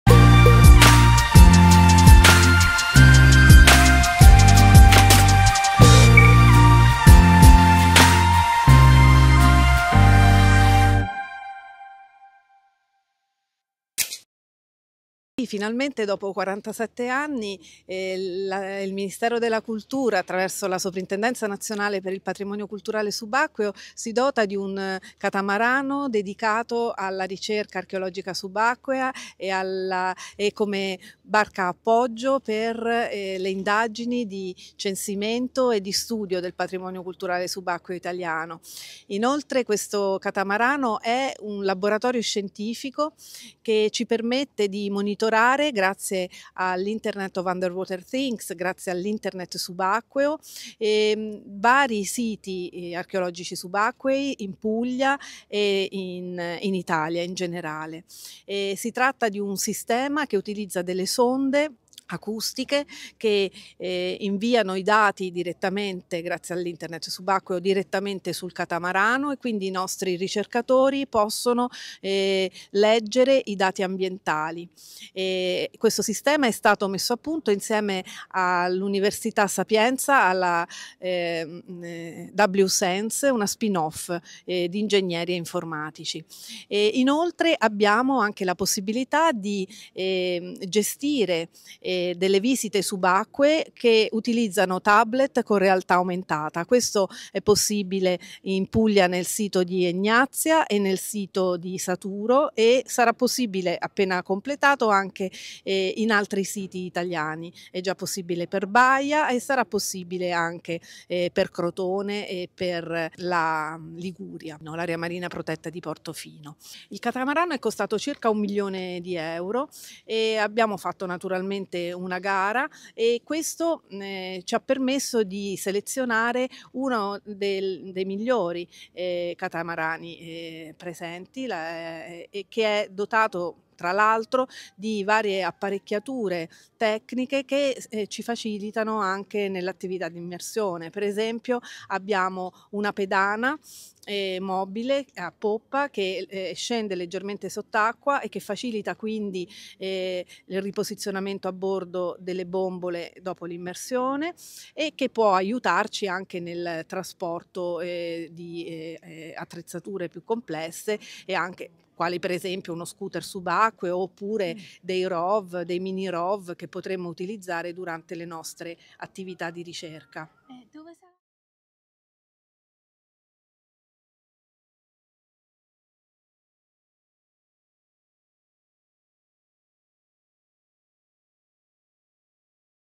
Catamarano per la ricerca archeologica subacquea "Amphitrite"
La Soprintendenza Nazionale per il patrimonio culturale subacqueo ha inaugurato il catamarano per la ricerca archeologica subacquea "Amphitrite" al Castello Aragonese di Taranto.